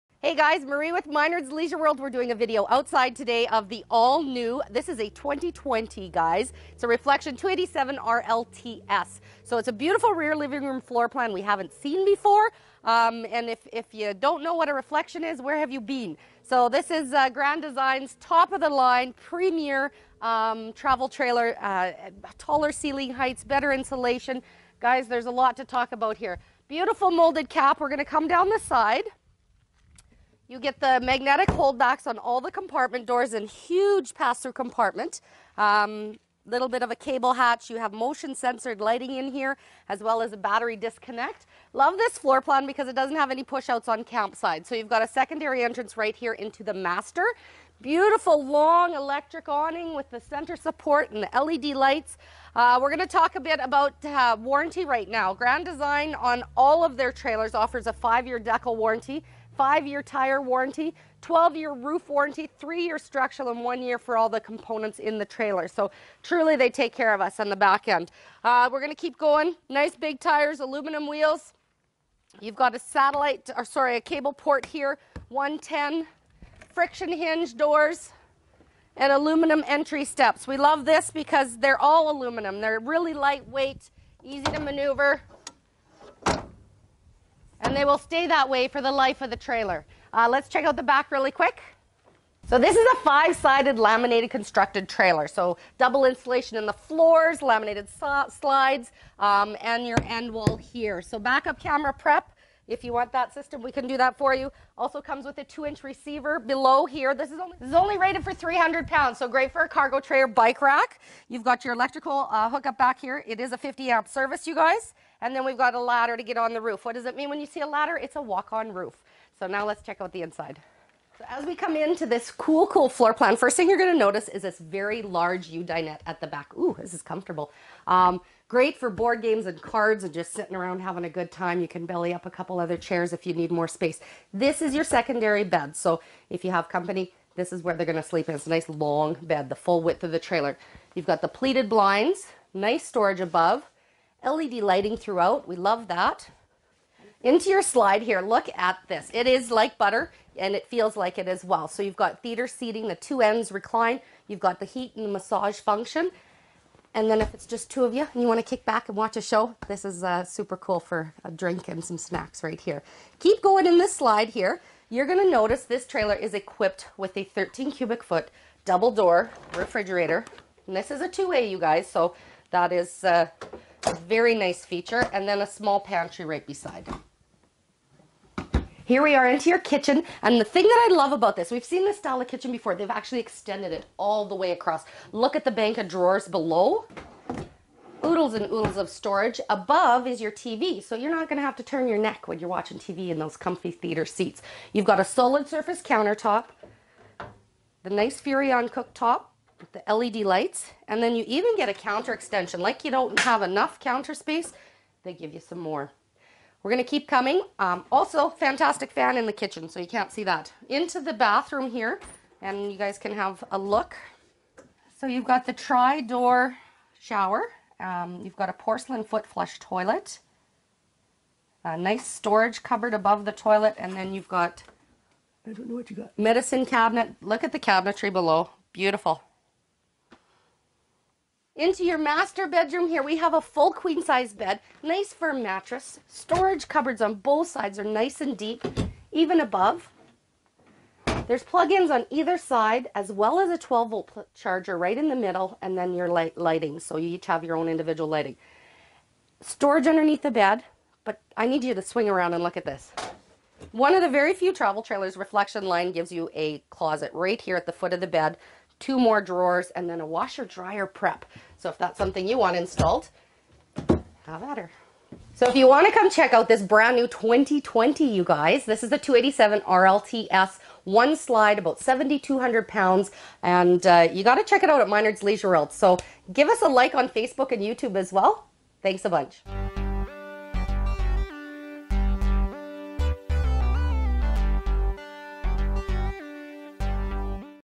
2020 Reflection 287RLTS by Grand Design
Travel trailer that looks like a million but won't tax the truck or break the bank!  Theatre seating, power jacks and super sized fridge make this Mom and Pop coach an instant winner!